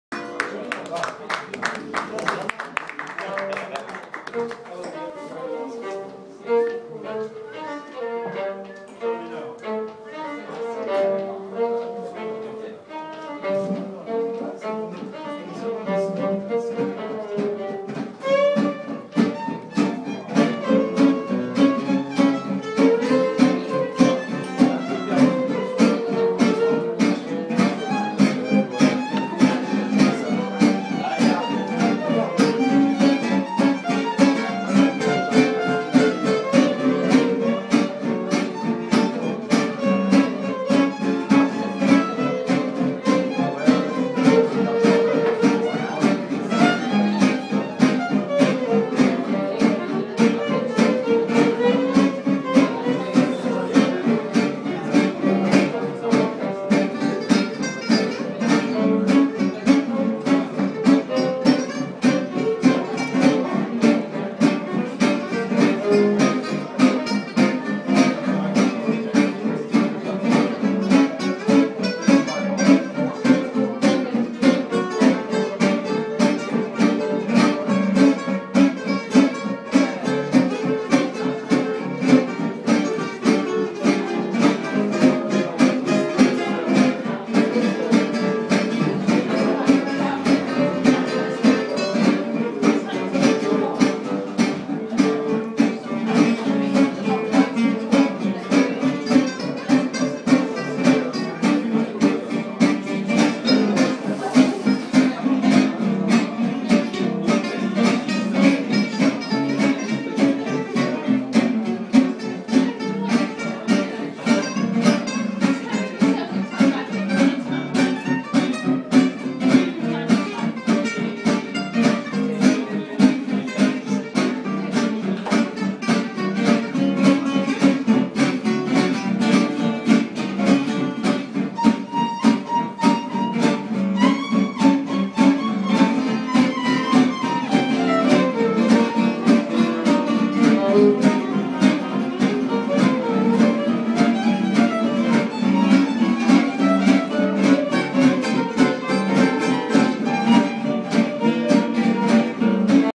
Gypsy Jazz - Bouquet Du Norde, Paris, France
House band plays here every Friday night.